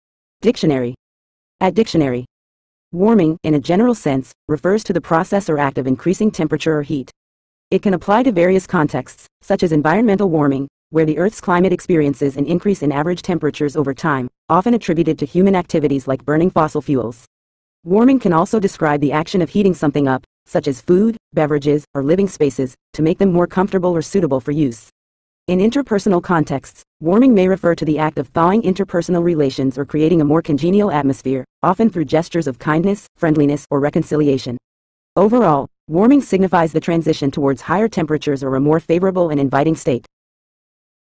Warming Meaning In English
Warming, in a general sense, refers to the process or act of increasing temperature or heat. It can apply to various contexts, such as environmental warming, where the Earth's climate experiences an increase in average temperatures over time, often attributed to human activities like burning fossil fuels. Warming can also describe the action of heating something up, such as food, beverages, or living spaces, to make them more comfortable or suitable for use. In interpersonal contexts, warming may refer to the act of thawing interpersonal relations or creating a more congenial atmosphere, often through gestures of kindness, friendliness, or reconciliation. Overall, warming signifies the transition towards higher temperatures or a more favorable and inviting state.